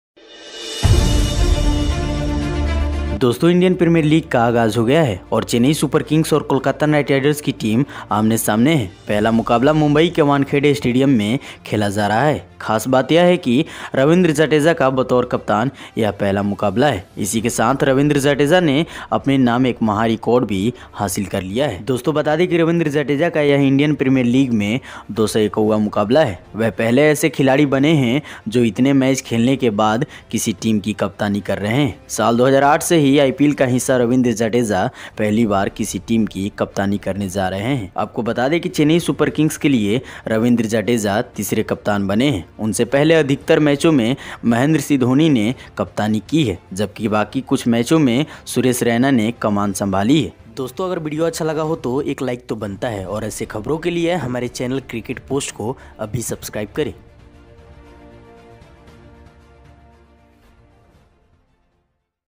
IPL 2022: Jadeja created history against KKR | kkr vs csk highlights| today match live |Cricket Post
IPL 2022: Jadeja created history against KKR | kkr vs csk highlights |today match live | Cricket Post. The Indian Premier League (IPL) has started and the teams of Chennai Super Kings-Kolkata Knight Riders (CSK-KKR) are face to face.  The first match is being played at Wankhede Ground in Mumbai, the special thing is that this is the first match of Ravindra Jadeja as captain.  With this, Ravindra Jadeja has also made a great record in his name.This is Ravindra Jadeja's 201st match in the Indian Premier League.  He has become the first player who is captaining a team after playing so many matches.  Since 2008, Ravindra Jadeja, a part of IPL, is going to captain a team for the first time.Let us tell you that Ravindra Jadeja has become the third captain for Chennai Super Kings.  Mahendra Singh Dhoni has captained most of the matches before him, while Suresh Raina has taken over the command in the remaining few matches.